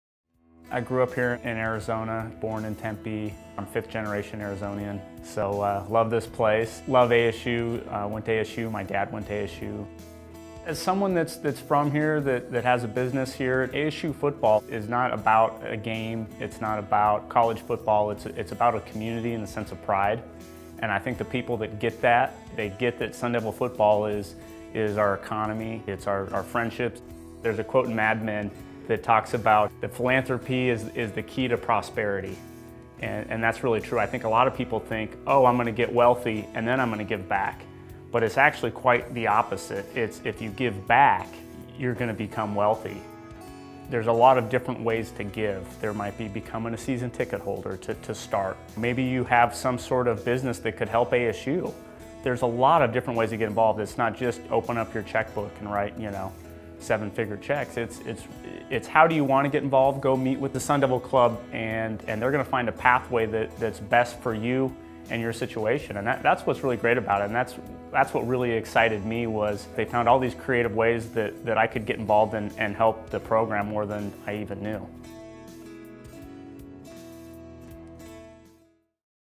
BeTheSolution: Scott Harkey, Alum
ASU alum Scott Harkey talks about contributing to the success of Arizona State through the Sun Devil Club.